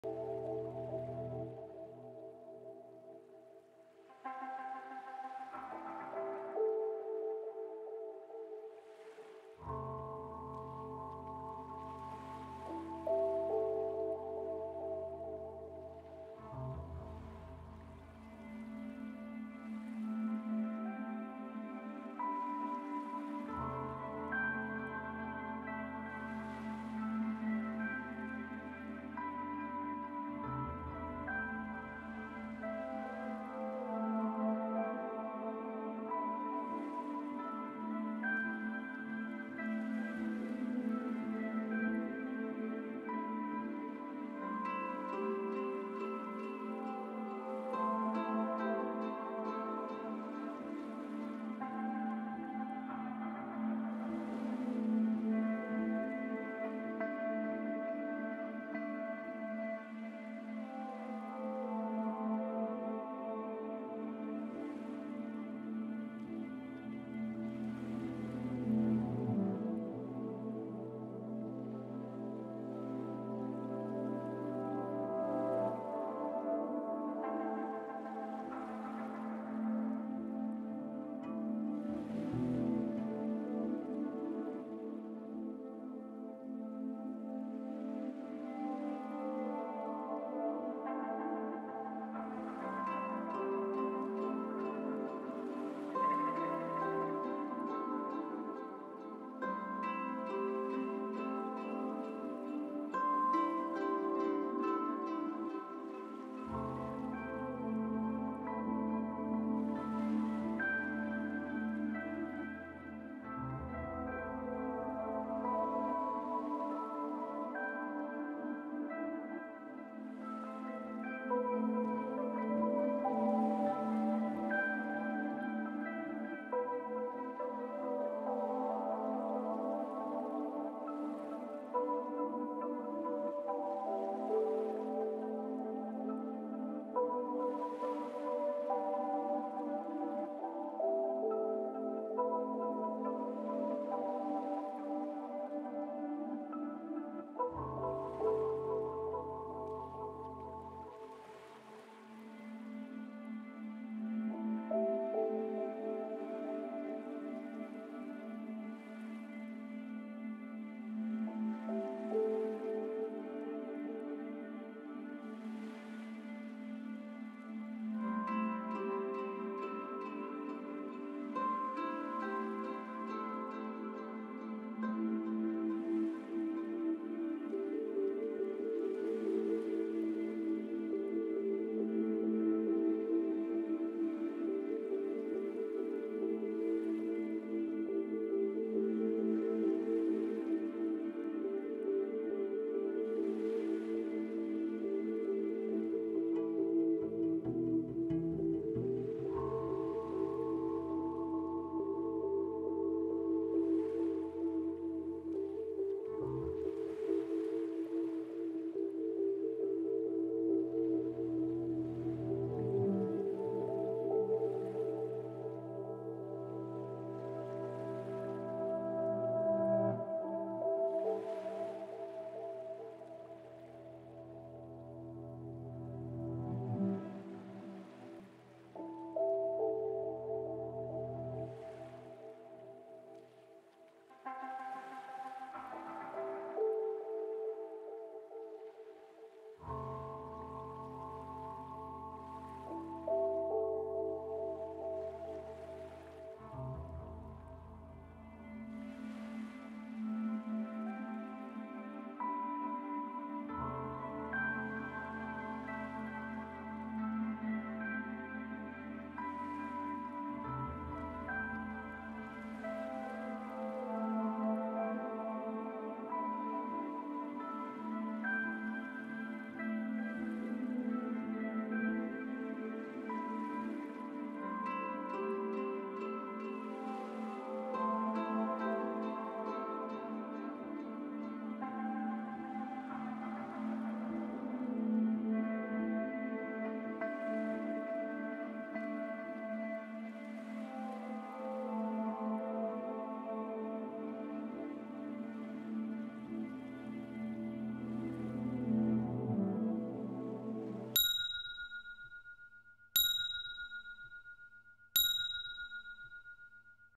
5 Minute Timer with Relaxing Music: Sunset Theme
A great countdown timer to use for anything!  Also, containing relaxing sunset videos!